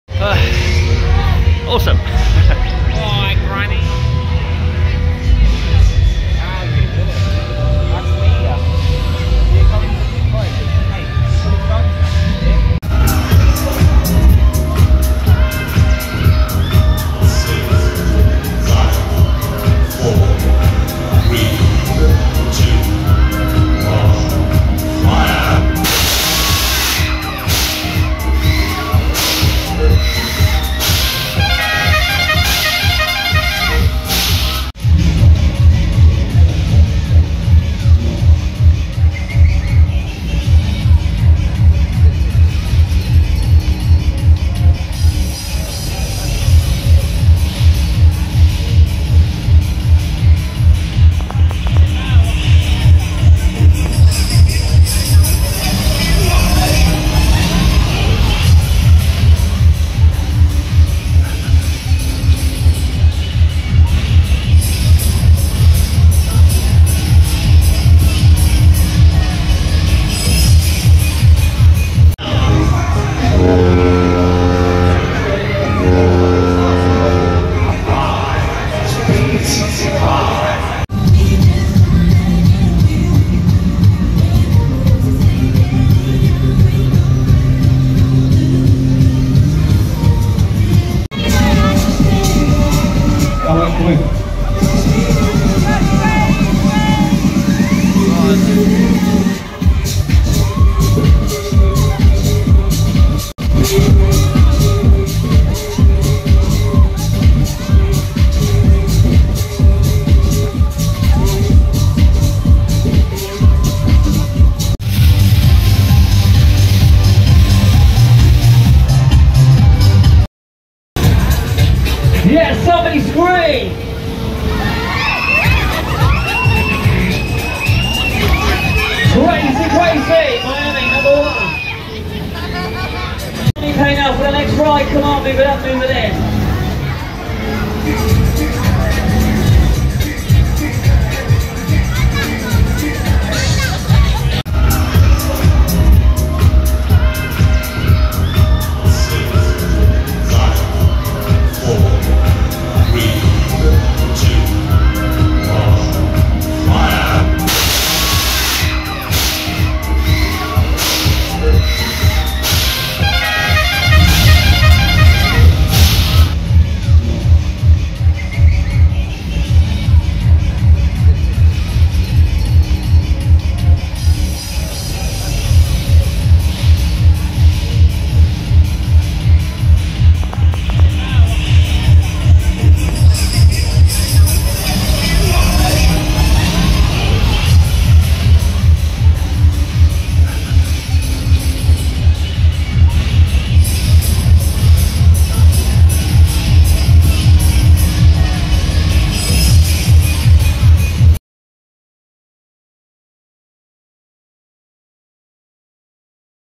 Loughborough Fun Fair November 2023 - From The Street and From above
See the fair from different angles at night, video and pics